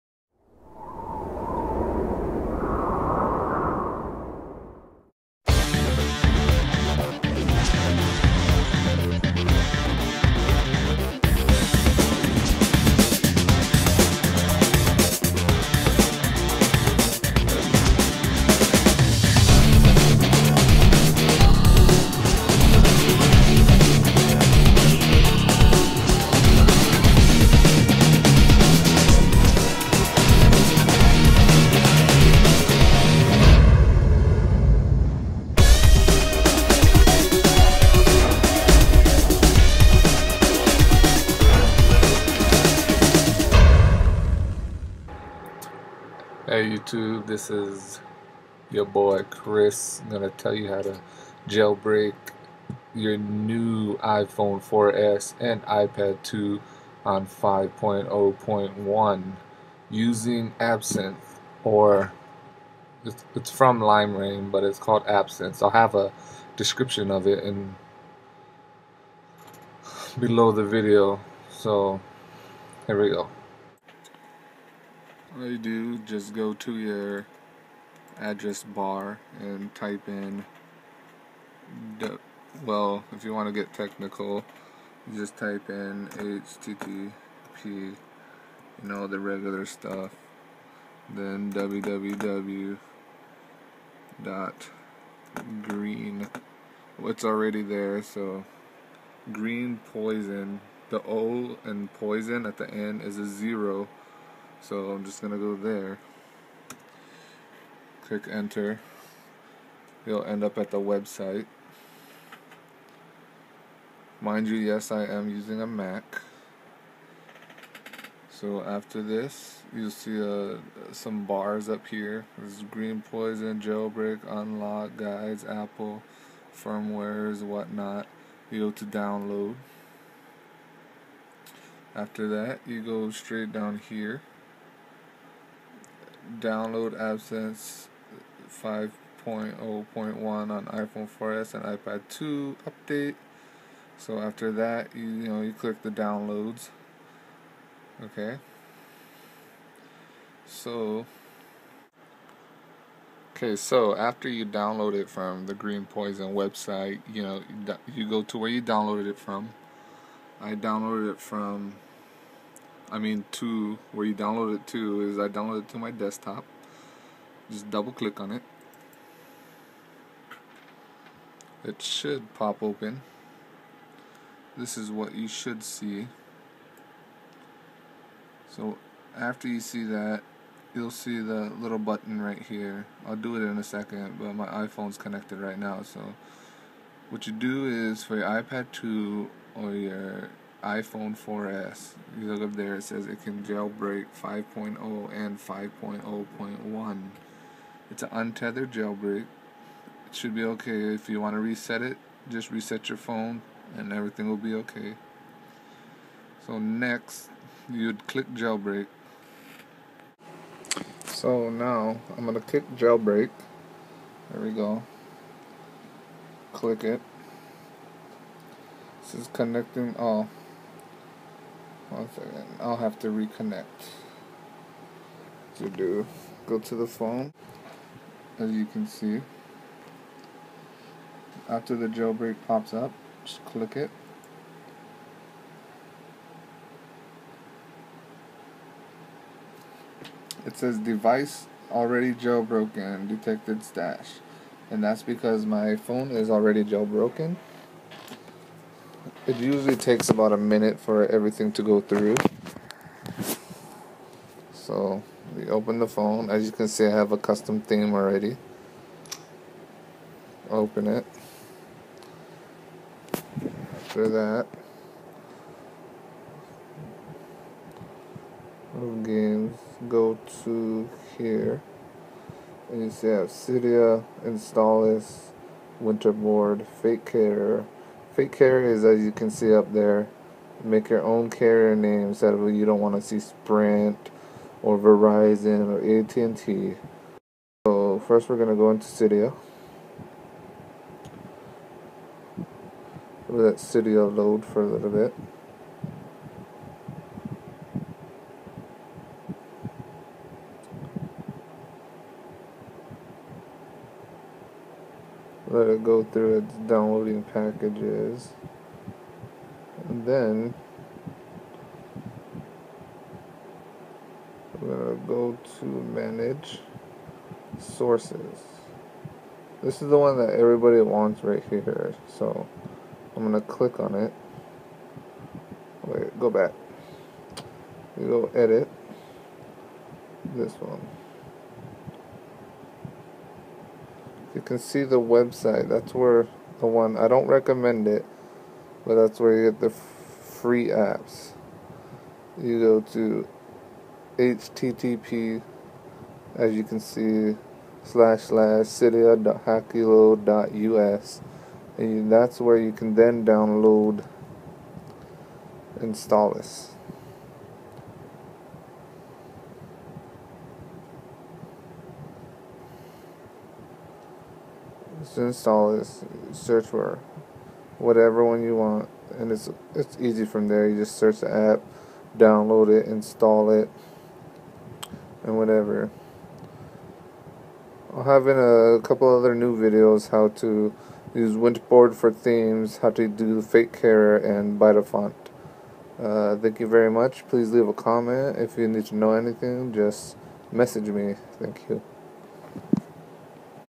Jailbreak iPhone 4S and iPad2 5.0.1
Showing everyone the info here is the website for the jailbreak program

and this is for the cydia info

before cydia is installed you will have to click on the absinthe icon on either your ipad2 or your iPhone 4S and cydia will then reset you device and you weill now have cydia

that is for the program to be able to install installous